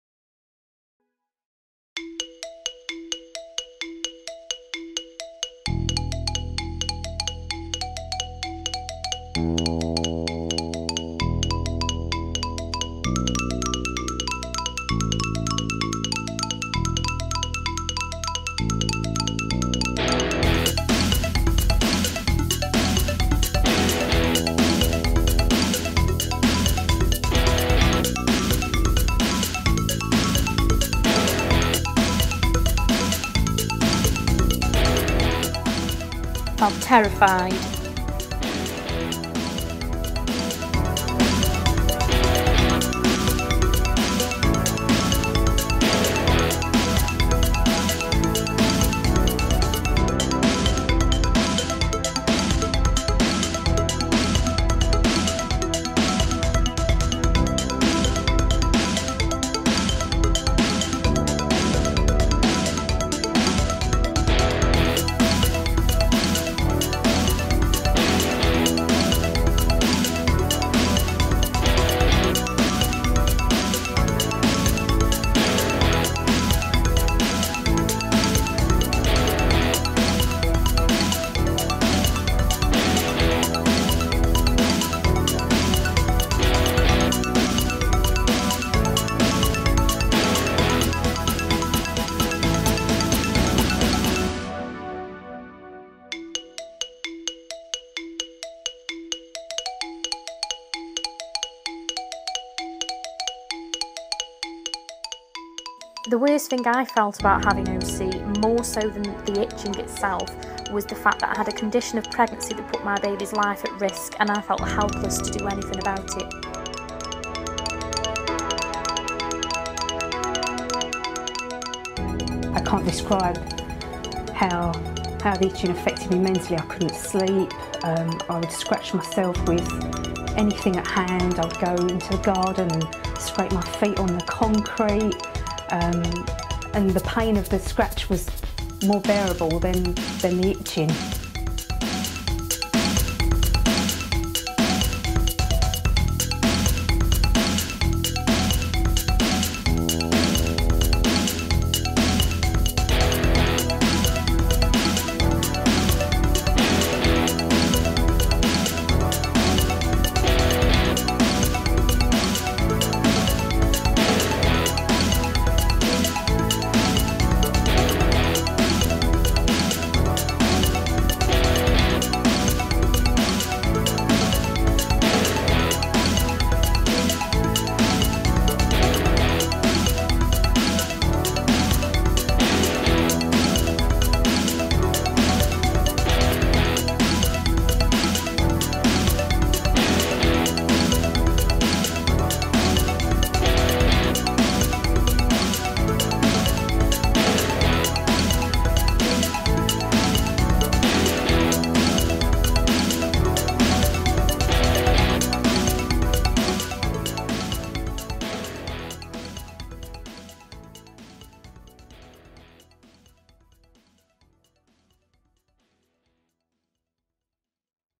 Living with Intrahepatic Cholestasis of Pregnancy
Women describe how intrahepatic cholestasis of pregnancy (ICP), also known as obstetric cholestasis (OC) affected them.

This video was first compiled to accompany a presentation made by Jenn Deasington and Jenny Chambers at an international conference on ICP held in London in September 2012.

The credit for making the film goes to Michael Kingston.